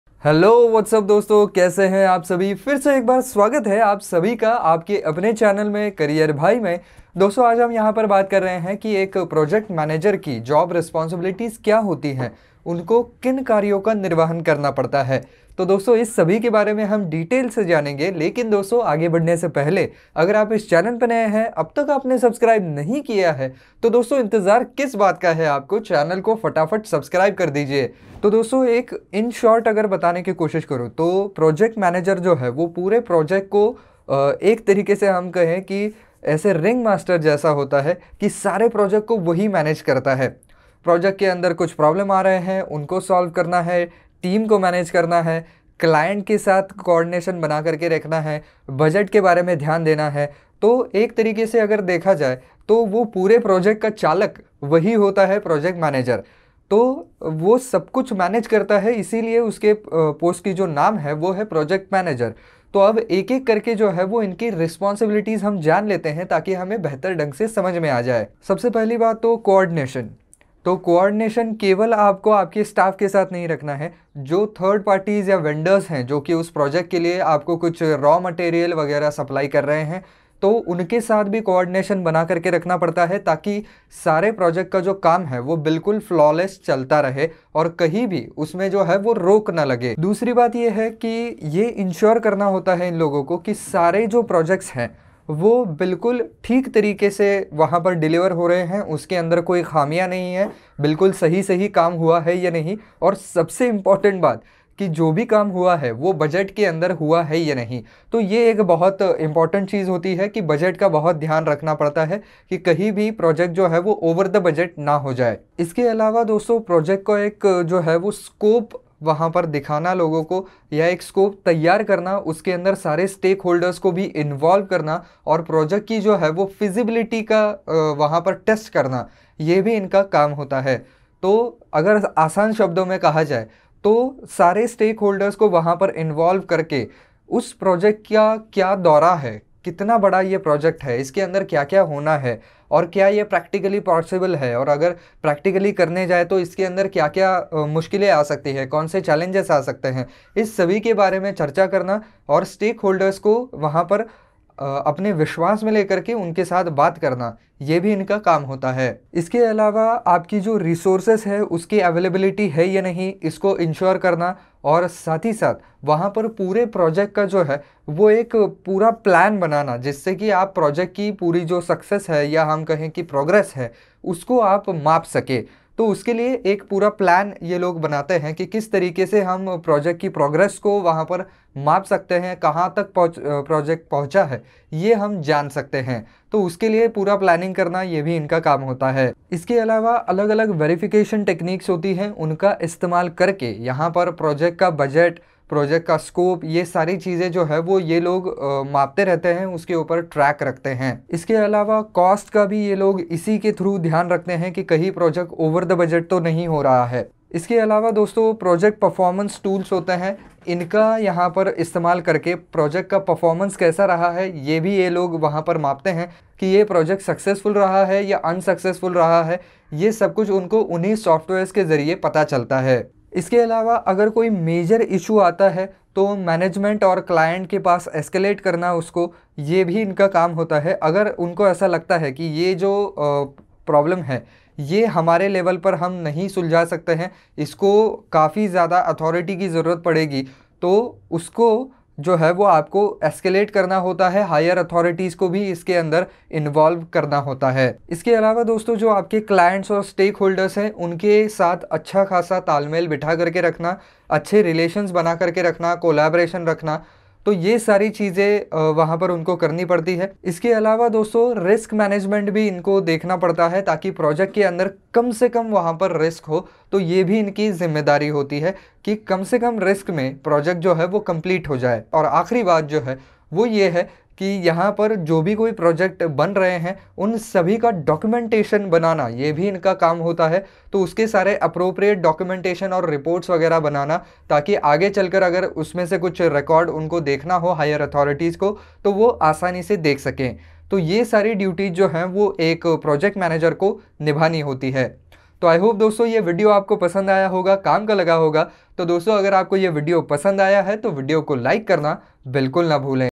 Project Manager Ke Role Aur Responsibilities - Duties of Project Manager
Project Manager ka role kya hota hai? Project Manager ki duties kya hoti hai? Project Manager ki jimmedariya kya hai? Is hindi video me maine ek Project manager ke functions aur duties ko bataya hai. Is video ko dekhkr aap ek project manager ke duties aur functions ko achi tarah samaj payenge.

What are the roles and responsibilities of a Project Manager? This video in Hindi explains the duties performed by the Project manager in a company. I've covered the functions performed by a Project manager in Hindi.

Project Manager duties, role and responsibilities explained in Hindi.